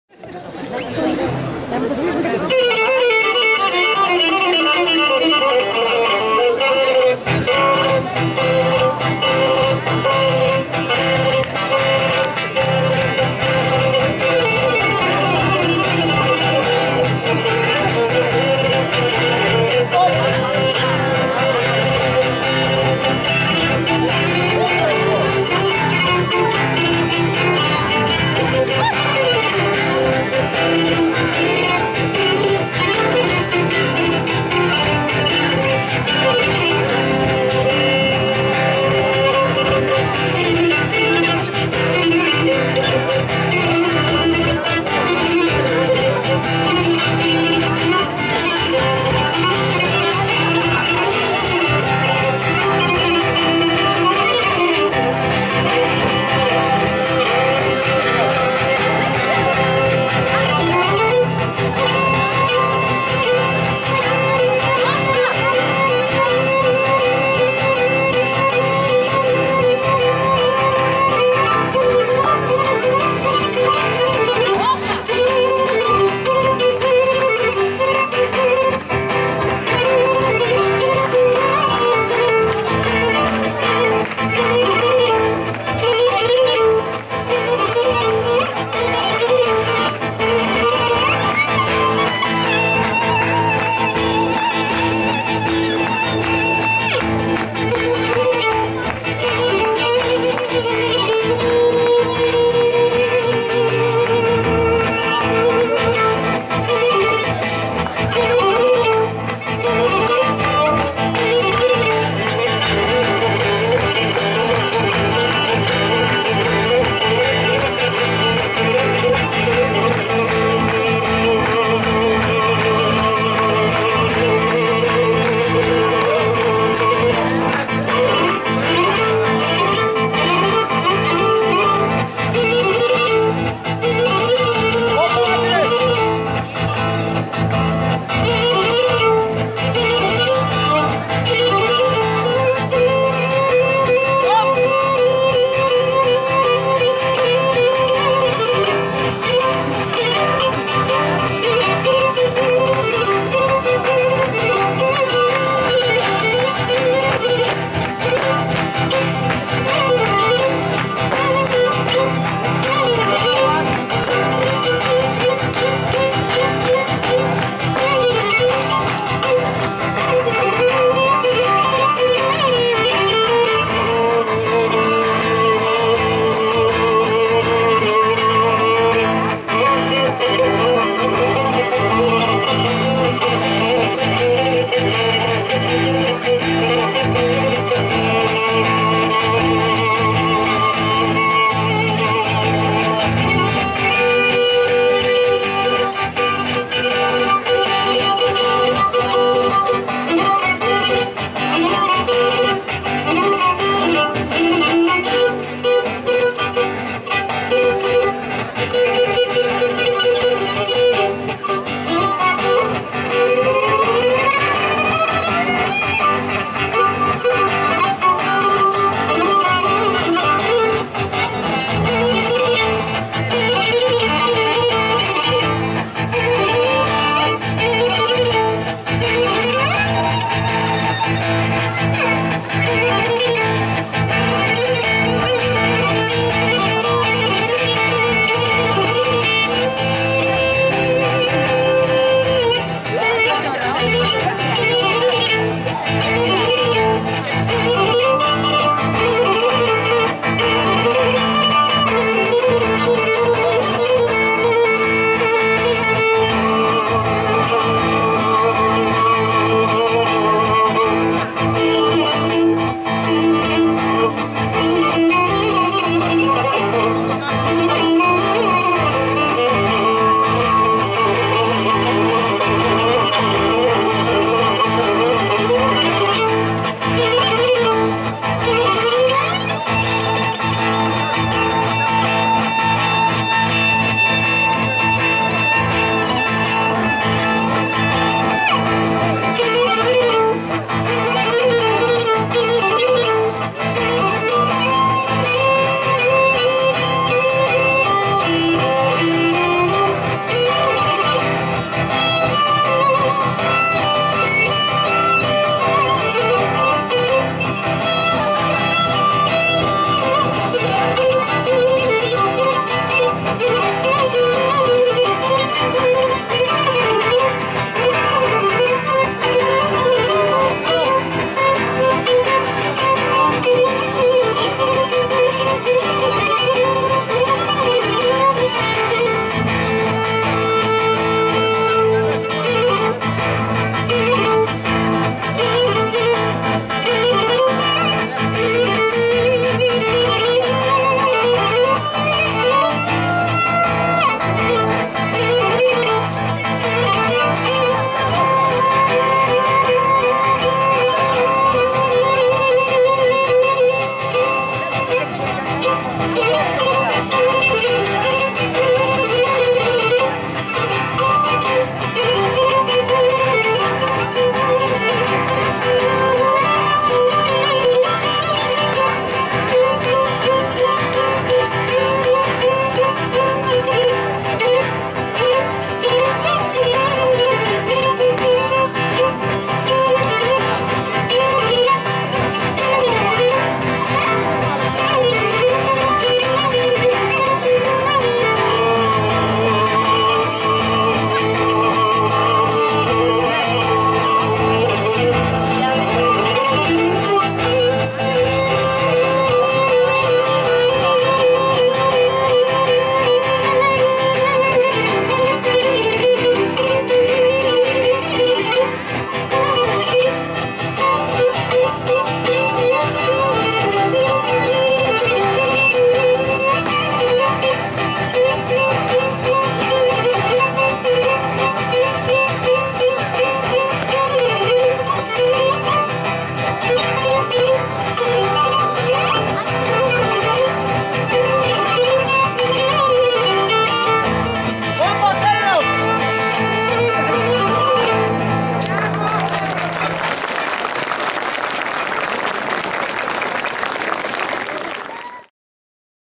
"ΤΟ ΞΑΘΕΡΙ",στα Κούλουμα του 2015 στην Παραλία Κατερίνης 3ο βίντεο
Καθαρά Δευτέρα!..."Κούλουμα 2015 στην Παραλία Κατερίνης" Συμμετοχή στην εκδήλωση του χορευτικού τμήματος του Εργαστηρίου Κρητικής Παράδοσης και Πολιτισμού Ν. Πιερίας "ΤΟ ΞΑΘΕΡΙ"